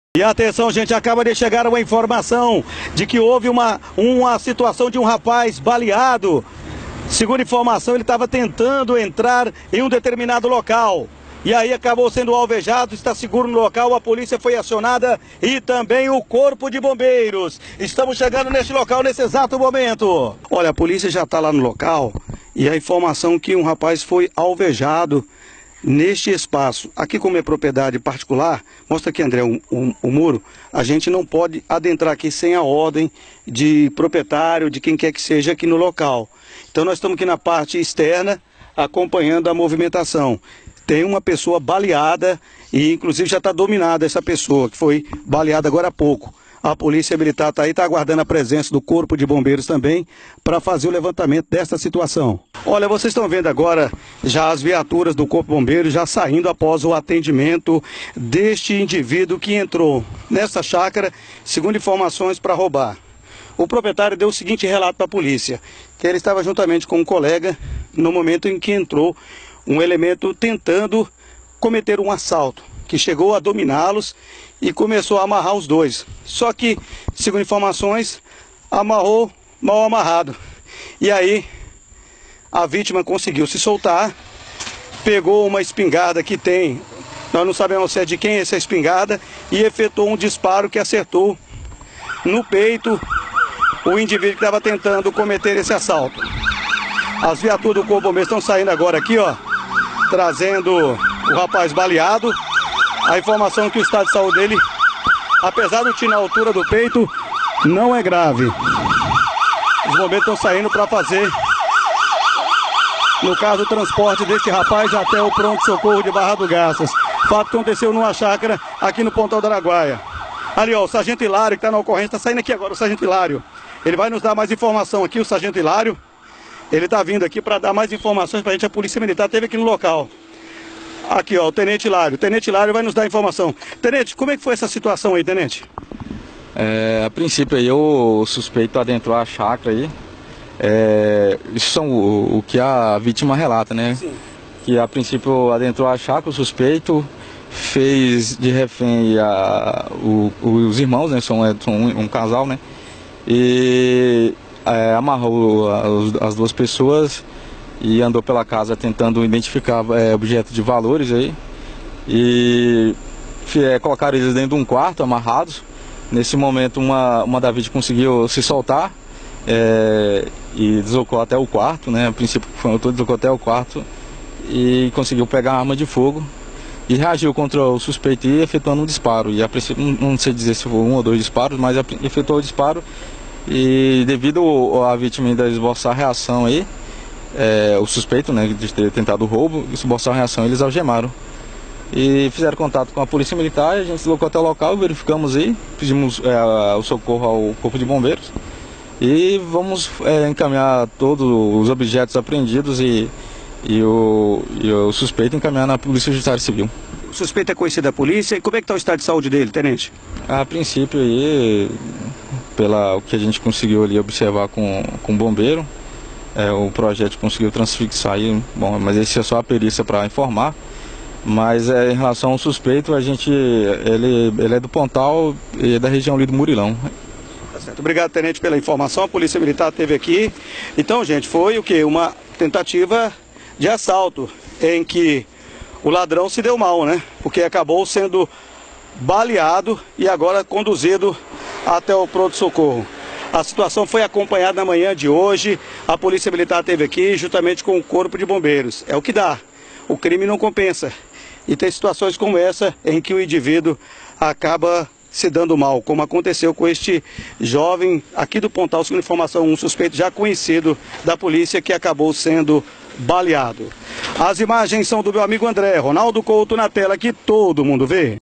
Assaltante é baleado ao invadir chácara
Pontal do Araguaia-MT